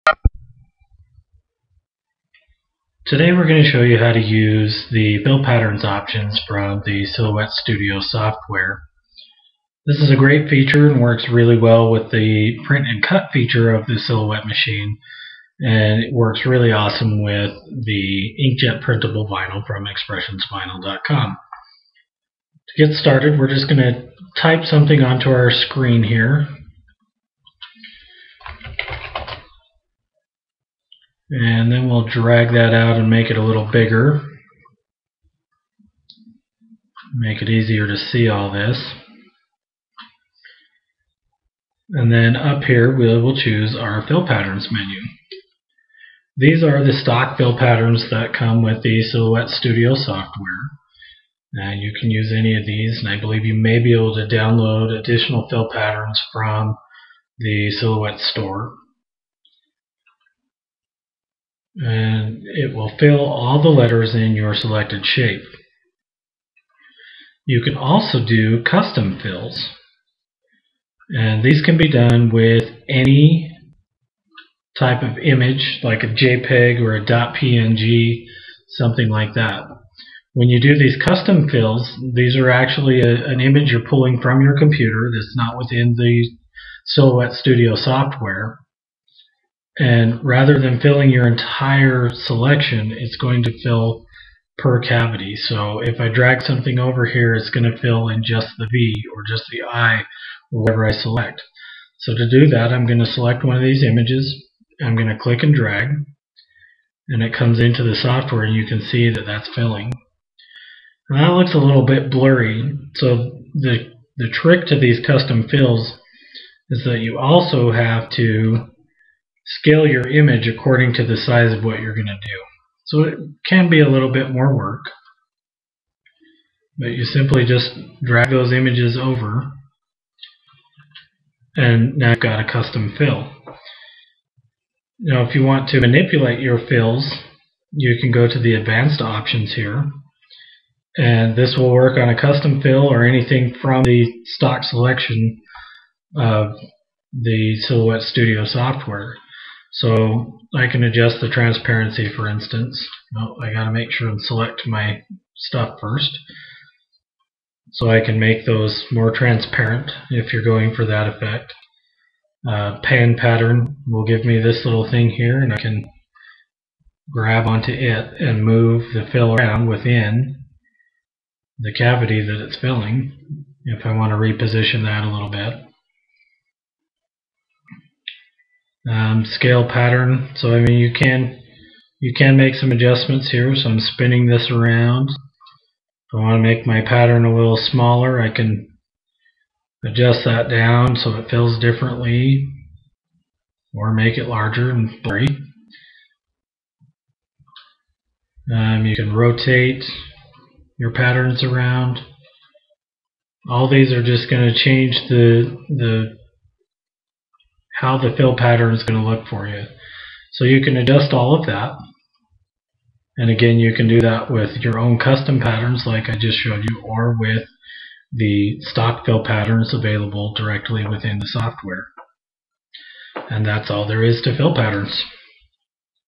Silhouette studio Designer Edition Fill Patterns
The Silhouette Cameo is here.  This fantastic new machine can accomplish many great things but today we want to showcase the Silhouette Studio designer edition software and it's ability to make patterned fills.

This is when you have a